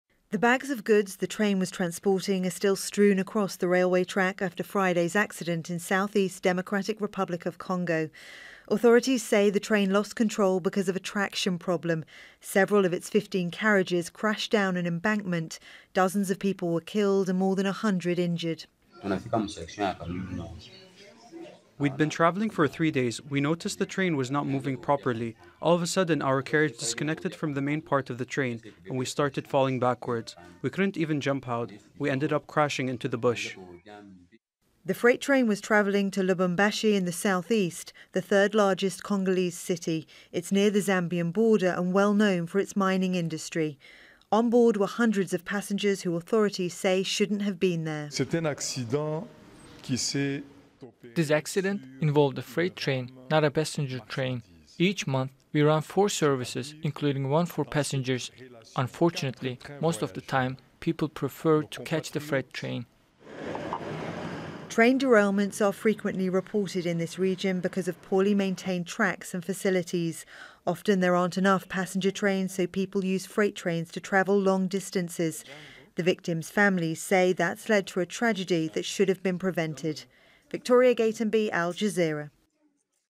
Dozens killed in freight train derailment in DR Congo
At least 75 people have now been confirmed dead after Friday's freight train derailment in the Democratic Republic of Congo.
It was travelling between two villages in the southern Lualaba province - before several carriages came off the tracks and crashed into a ravine.
Because there are not enough passenger trains from the region, people often use the freight trains to travel long distances.